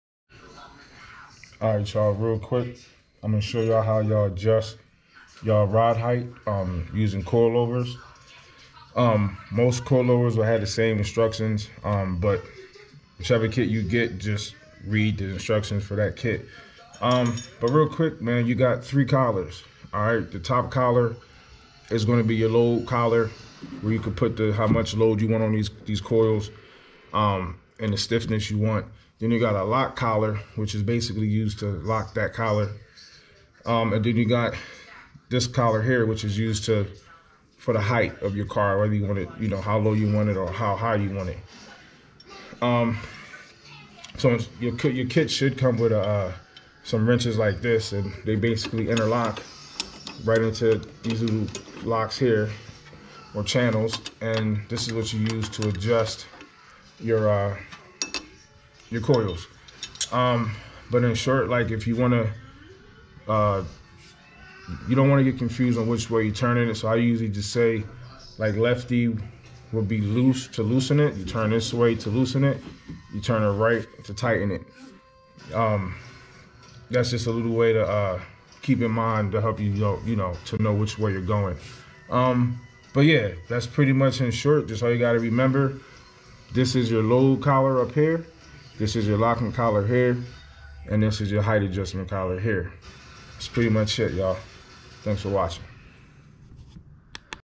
How to adjust Coilovers
In this video I show you how to adjust Coilovers

If you Enjoyed the video or if the video helped you consider contributing to the channel👍